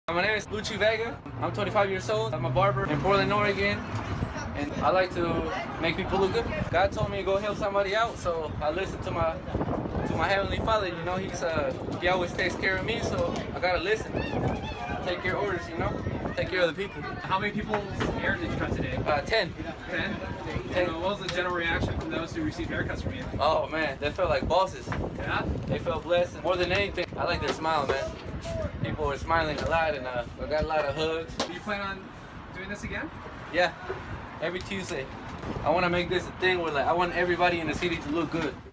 Changing the Portland Community One Haircut at a Time
Luchie Vega cuts hair for the homeless population on Tuesdays in front of UGM.